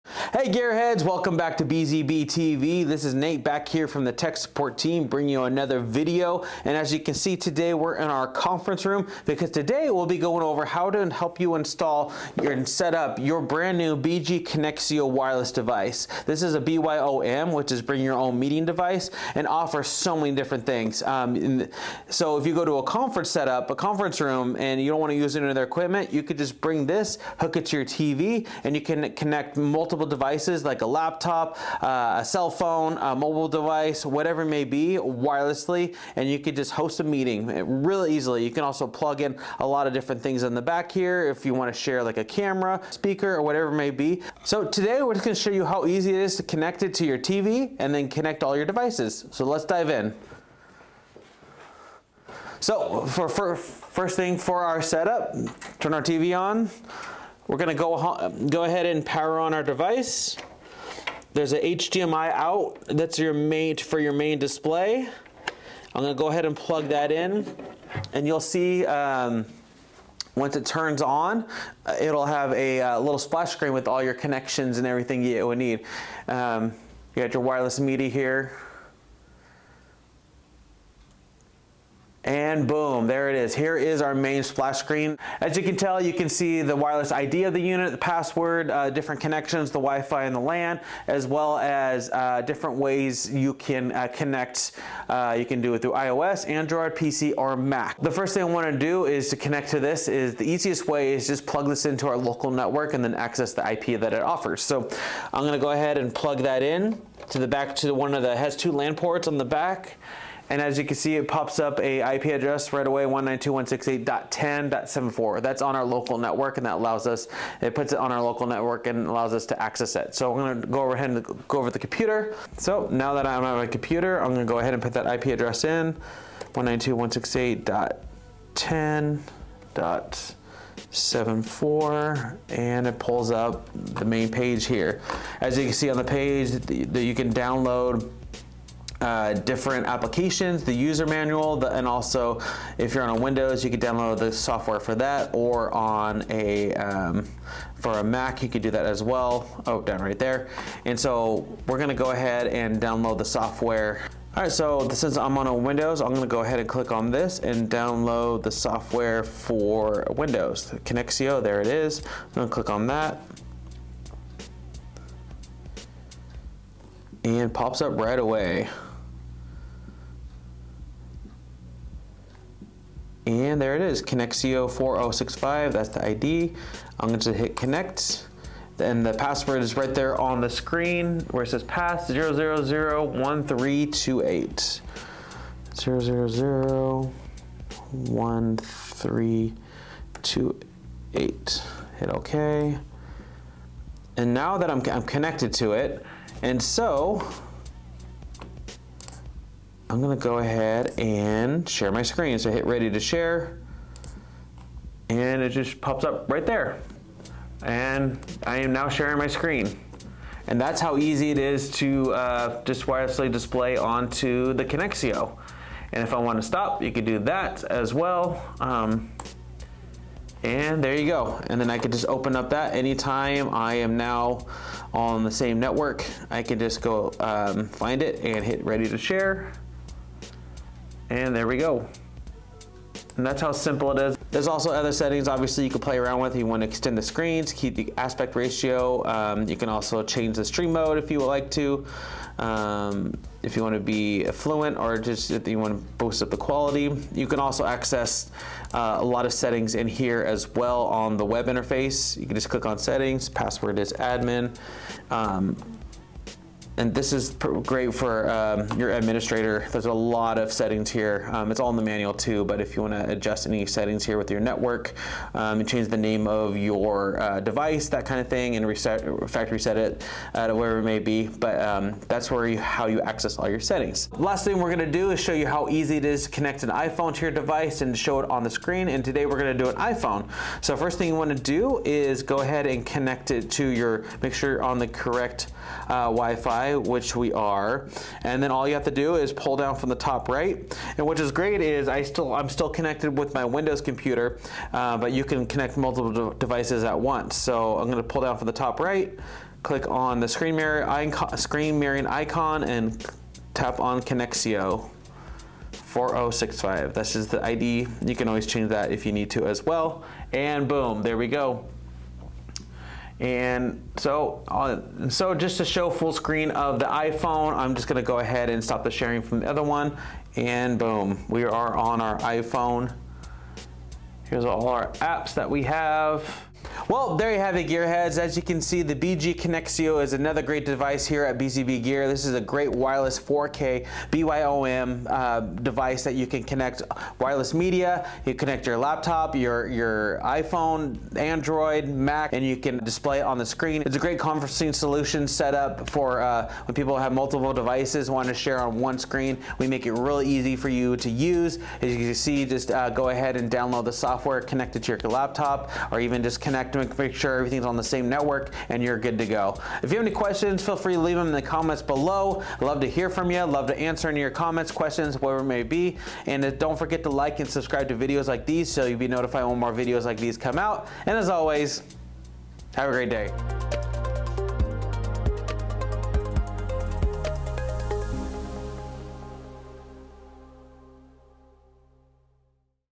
Easy Set-up Wireless Collaboration & Conference System - BG-Connexio
The BG-Connexio can manage any conference A/V system from your personal device, allowing you to adjust the volume, change the source, or even control the camera and microphone as needed. This flexibility means that you can create an immersive and collaborative meeting experience for all attendees, whether they are physically present or participating remotely.

Product Link